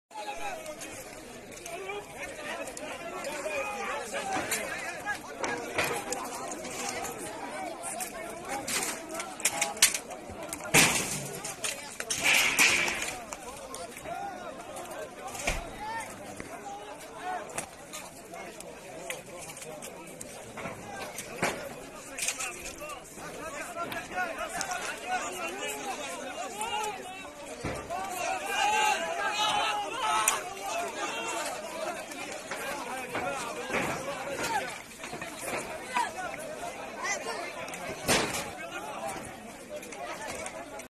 15 قتيلا فلسطينيا بقصف إسرائيلي على مدينة خان يونس
قصف إسرائيلي جديد على مدينة خان_يونس في قطاع غزة يوقع حوالي 15 قتيلا أغلبهم من الأطفال بحسب مصادر محلية
لمعرفة أحدث المستجدات.. عبر البث المباشر من هنا ⤵
وبإمكانكم متابعة الحدث على منصات التواصل